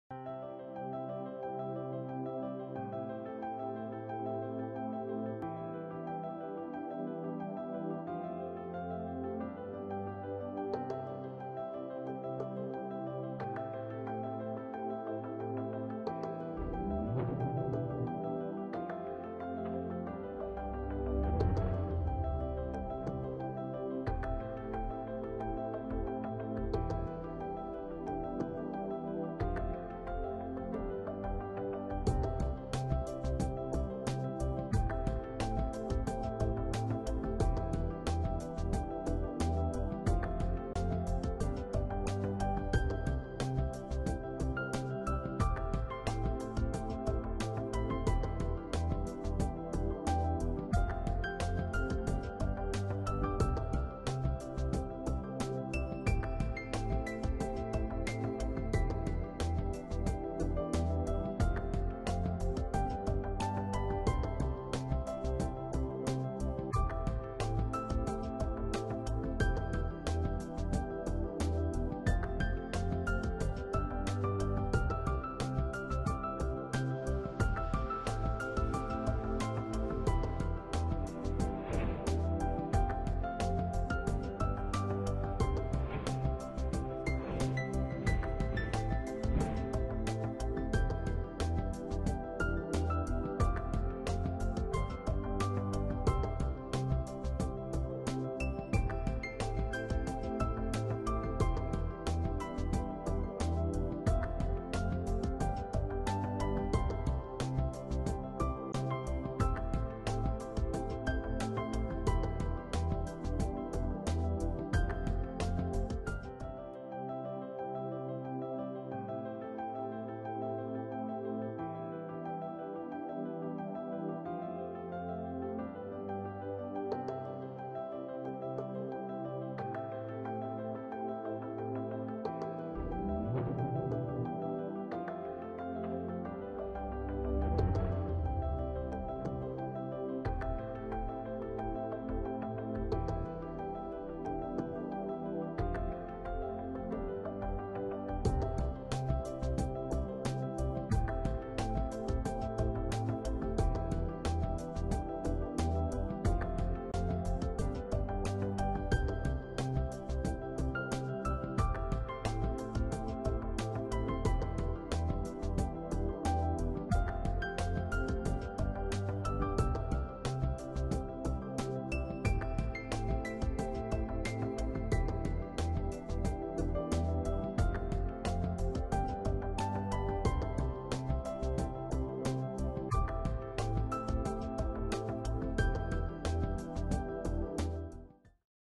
Dioxin, PCB and PBDE Sample Clean-Up
EZprep 123™ Semi-Automated System - Simple, Quick & Low Cost Sample Cleanup for Dioxin, PCB and PBDE Analysis